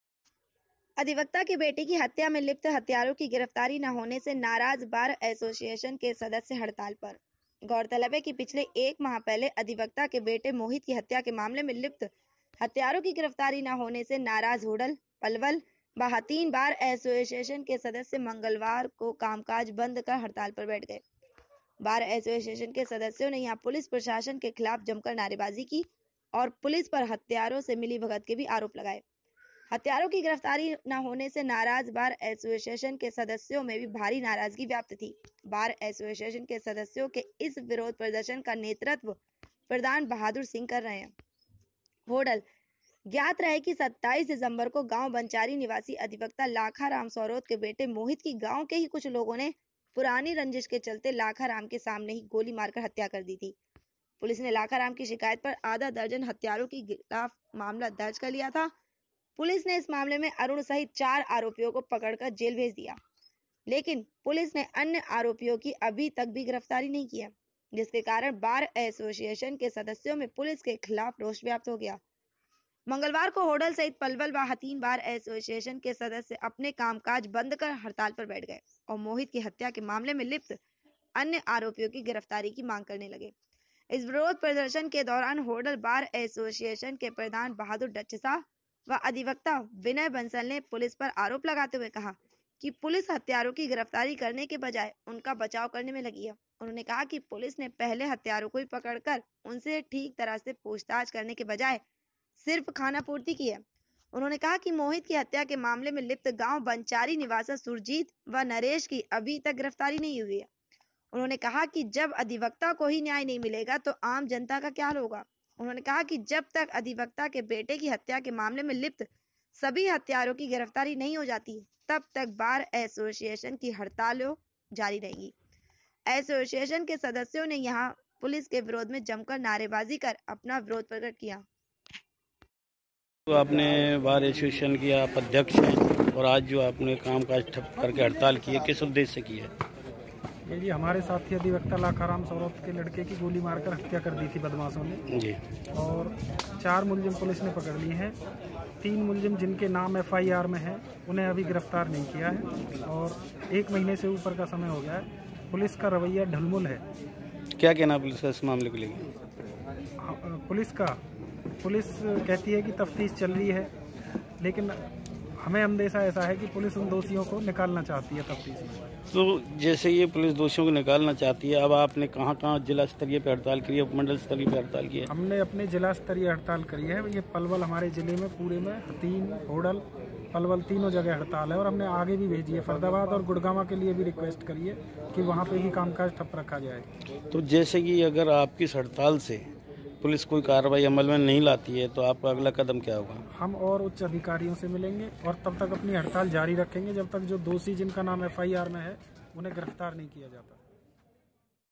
अधिवक्ता के बेटे के हत्यारों को गिरफ्तार न करने पर बार एसोसिएशन के सदस्य हड़ताल पर
अधिवक्ता के बेटे की हत्या में लिप्त हत्यारों की गिरफ्तारी ना होने से नाराज बार एसोसिएशन के सदस्य हडताल पर

गौरतलब है की पिछले 1 महा पहले अधिवक्ता के बेटे मोहित की हत्या के मामले में लिप्त हत्यारों की गिरफ्तारी ना होने से नाराज होडल, पलवल व हथीन बार एसोसिएशन के सदस्य मंगलवार को कामकाज बंद कर हडताल पर बैठ गए। बार एसोसिएशन के सदस्यों ने यहां पुलिस प्रशासन के खिलाफ जमकर नारेबाजी की और पुलिस पर हत्यारों से मिलीभगत के भी आरोप लगा। हत्यारों की गिरफ्तारी ना होने से नाराज बार एसोसिएशन के सदस्यों में भारी नाराजगी व्याप्त थी। बार एसोसिएशन के सदस्यों के इस विरोध प्रदर्शन का नेतृत्व प्रधान बहादुर  सिंह  कर रहे है

होडल ज्ञात रहे कि 27 दिसंबर को गांव बंचारी निवासी अधिवक्ता लाखाराम सौरोत के बेटे मोहित की गांव के ही कुछ लोगों ने पुरानी रंजिश के चलते लाखाराम के सामने ही गोली मारकर हत्या कर दी थी। पुलिस ने लाखाराम की शिकायत पर आधा दर्जन हत्यारों की खिलाफ मामला दर्ज कर लिया था। पुलिस ने इस मामले में अरूण सहित चार आरोपियों को पकडक़र जेल भेज दिया, लेकिन पुलिस ने अन्य आरोपियों की अभी तक भी गिरफ्तारी नहीं किया है। जिसके कारण बार एसोसिएशन के सदस्यों में पुलिस के खिलाफ रोष व्याप्त हो गया। मंगलवार को होडल सहित पलवल व हथीन बार एसोसिएशन के सदस्य अपने कामकाज बंद कर हडताल पर बैठ गए और मोहित की हत्या के मामले में लिप्त अन्य आरोपियों की गिरफ्तारी की मांग करने लगे। इस विरोध प्रदर्शन के दौरान होडल बार एसोसिएशन के प्रधान बहादुर ङ्क्षसह व अधिवक्ता विनय बंसल ने पुलिस पर आरोप लगाते हुए कहा कि पुलिस हत्यारों की गिरफ्तारी करने के बजाय उनका बचाव करने में लगी है। उन्होंने कहा कि पुलिस ने पहले हत्यारों को भी पकडकर उनसे ठीक तरह से पूछताछ करने के बजाय सिर्फ खानापूर्ति की है।  उन्होंने कहा कि मोहित की हत्या के मामले में लिप्त गांव बंचारी निवासर सुरजीत व नरेश की अभी तक गिरफ्तारी नहीं हुई है। उन्होंने कहा कि जब अधिवक्ता को ही न्याय नहीं मिलेगा तो आम जनता का क्या हाल होगा। उन्होंने कहा कि जब तक अधिवक्ता के बेटे की हत्या के मामले में लिप्त सभी हत्यारों की गिरफ्तारी नहीं हो जाती तब तक बार एसोसिएशन की हडताल यों ही जारी रहेगी। एसोसिएशन के सदस्यों ने यहां पुलिस के विरोध में जमकर नारेबाजी कर अपना विरोध प्रकट किया।